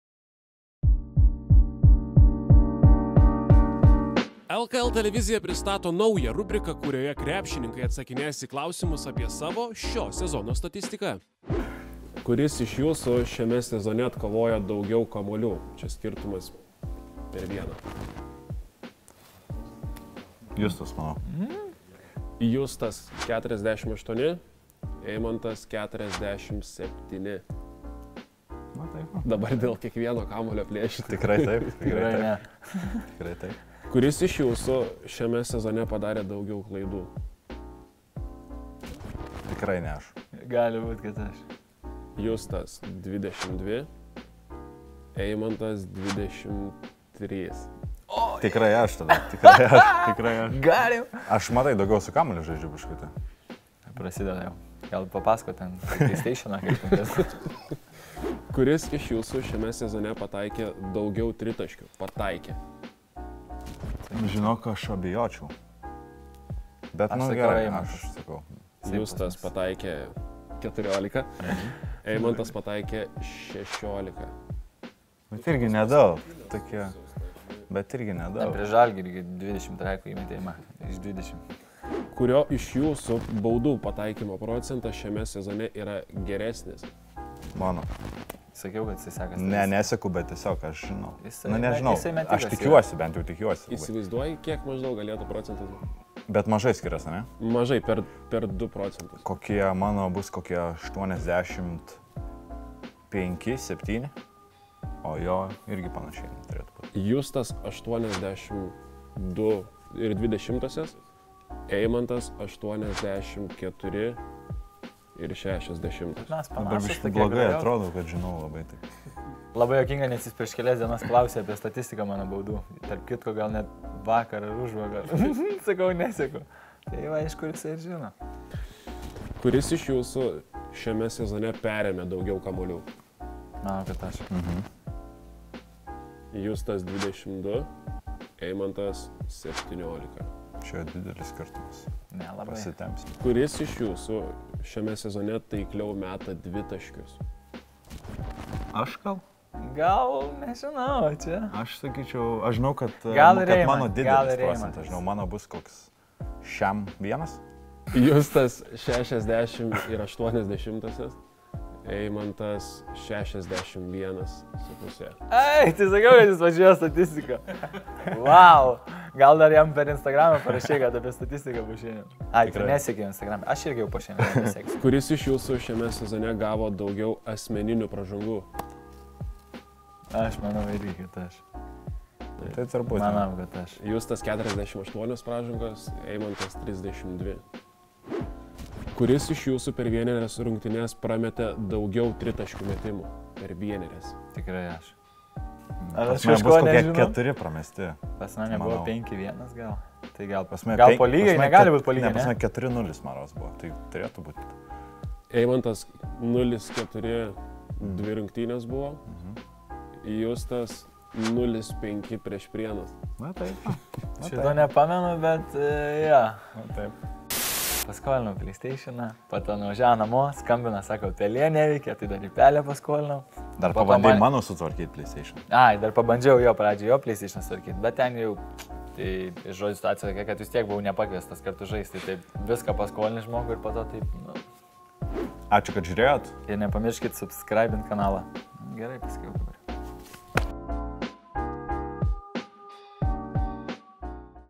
„Vienas ant vieno“: kuris iš „Šiaulių“ krepšininkų geriau išmano statistiką?
LKL TV rubrikoje „Vienas ant vieno“ šįkart varžėsi „Šiaulių“ krepšininkai – Eimantas Stankevičius ir Justas Furmanavičius.

Saulės miesto ekipos atstovai rungėsi, kuris iš jų geriau išmano tiek vienas kito, tiek savo pačių statistines eilutes.

„Sakau, kad jis pažiūrėjo statistiką“, – po to, kai komandos draugas tiksliai įvardino savo dvitaškių pataikymo procentą, skundėsi J. Furmanavičius.